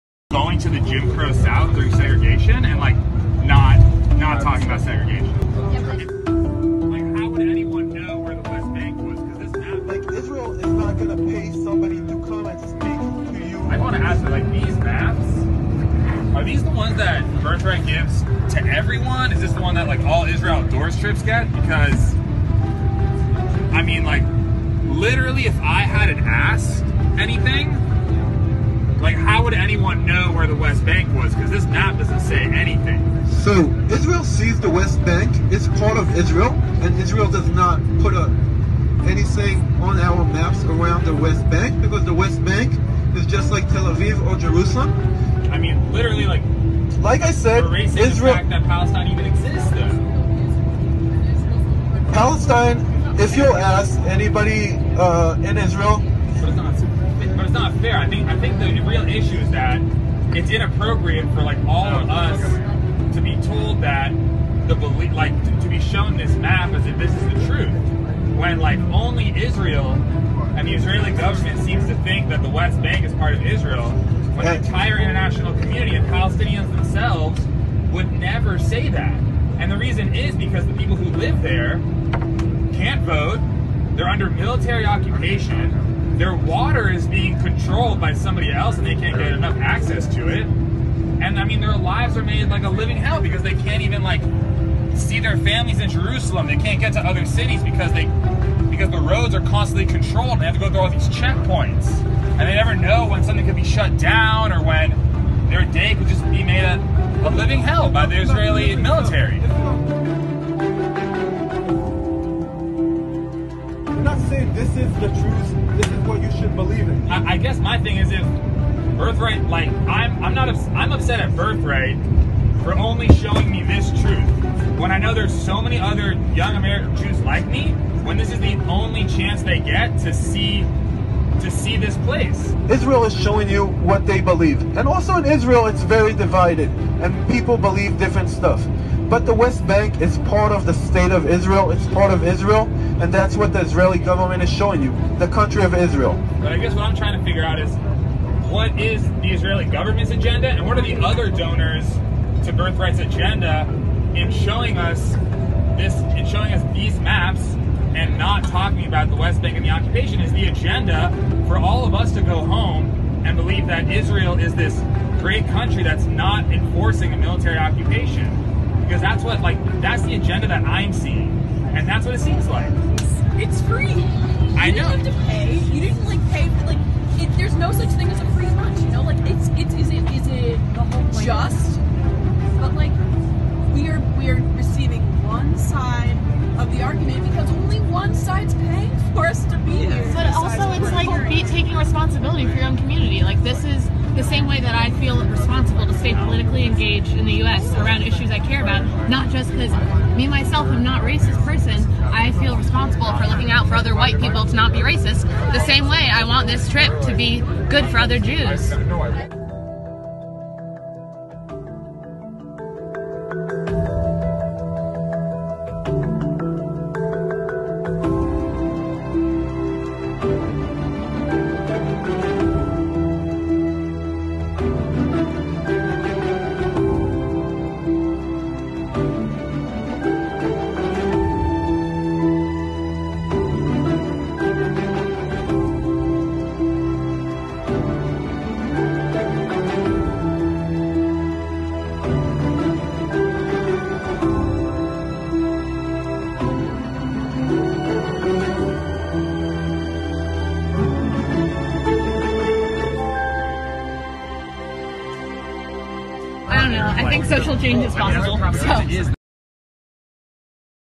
"Speak Up, Stand Up!" 🇵🇸🇵🇸🇵🇸
Elon Glickman, an American Jew, speaks out during a tour of "Palestine" when the Tour Operator, Birthright, tried hiding the fact that Palestine actually exists! 👏🏽👏🏽👏🏽